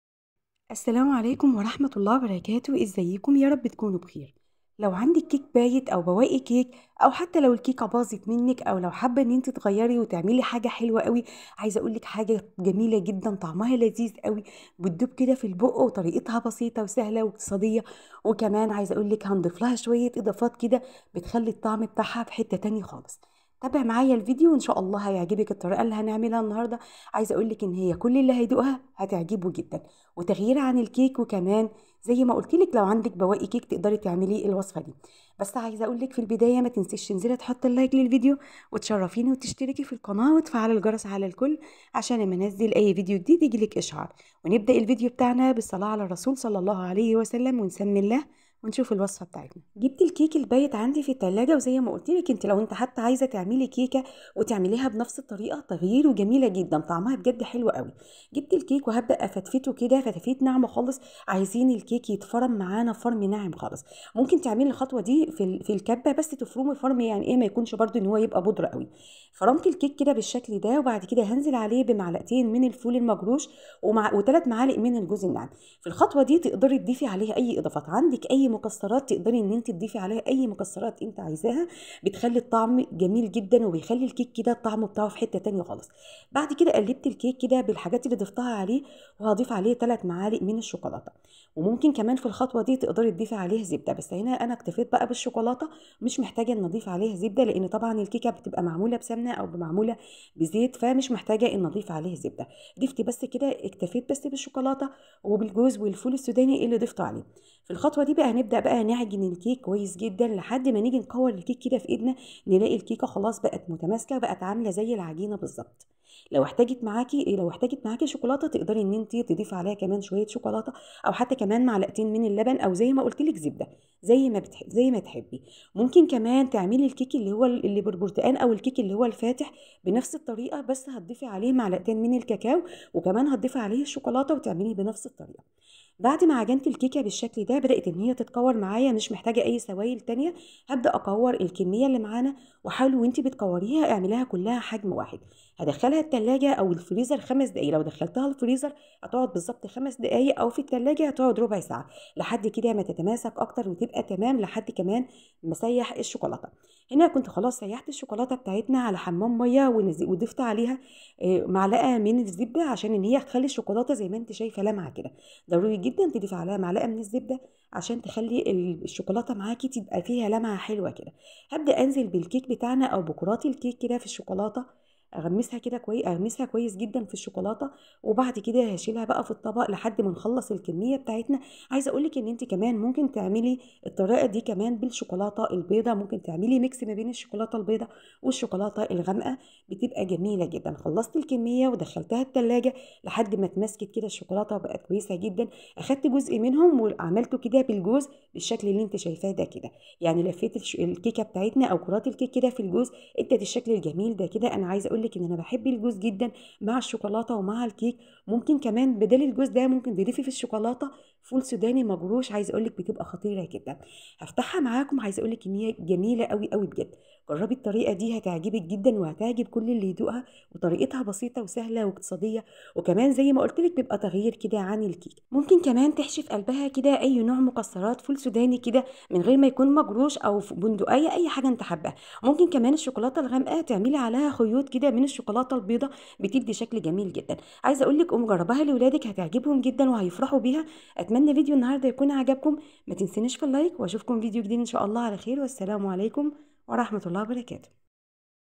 كرات الشيكولاته اللذيذه كيك بوبس⁉️ب3مكونات فقط وفى 10دقائق ومن بقايا الكيك بطعم لا يقاوم
ببواقى الكيك هنعمل كيك بويس
الذ كورات شيكولاته بناكهه مختلفه
كورات الشيكولاته الاقتصاديه